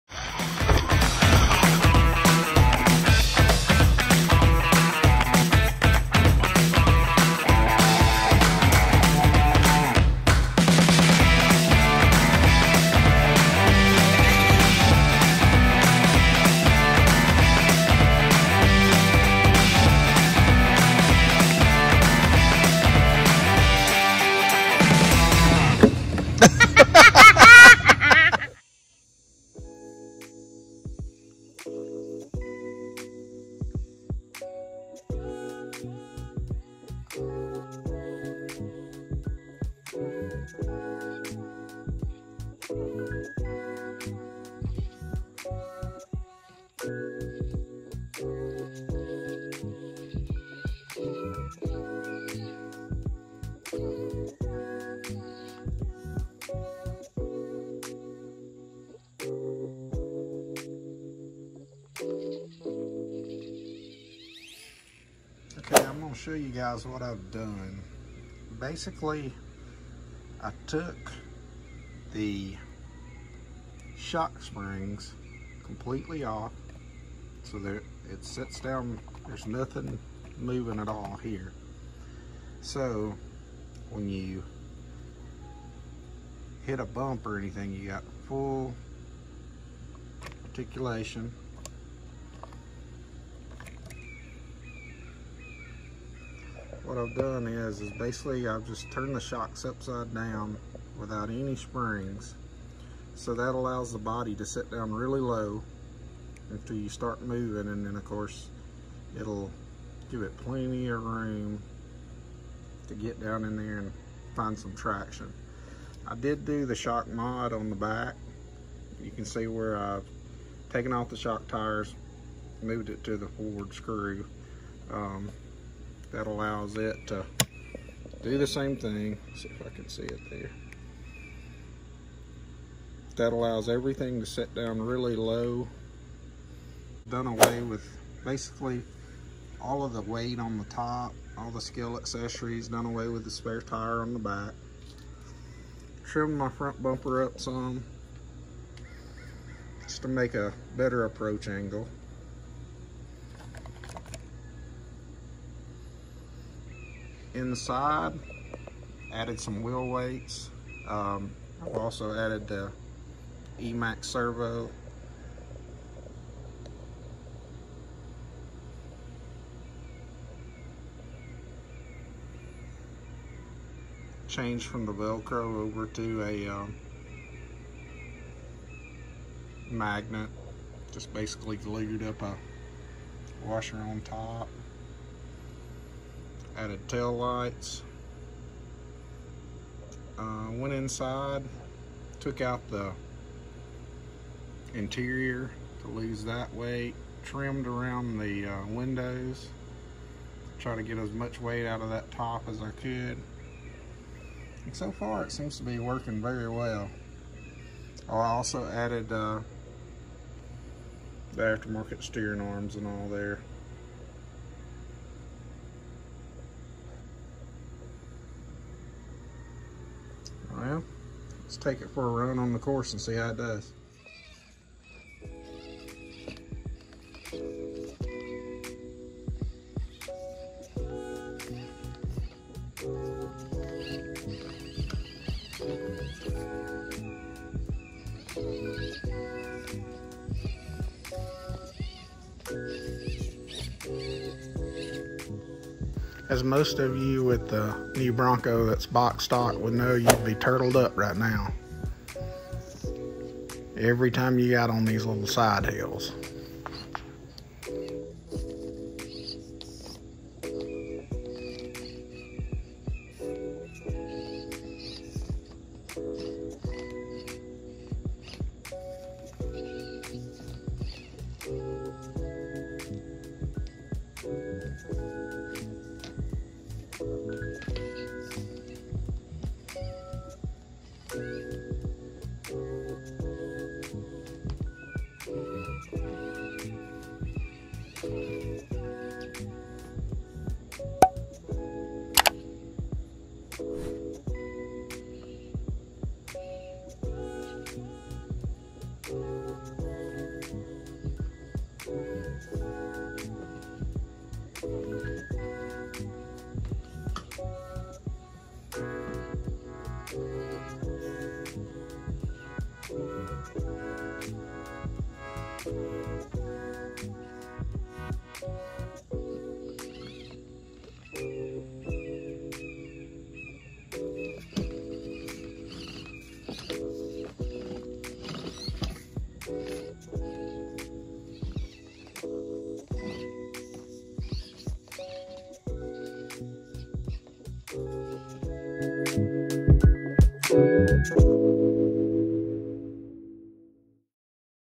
Axial SCX24 Ford Bronco Best 💢FREE💢 Shock upgrade
Axial SCX24 Best 💢FREE💢 Shock upgrade for the New Ford Bronco with teat run.
Also please drop a comment If you like this Idea or If there Is a negative to this mob. Thanks for your Input!!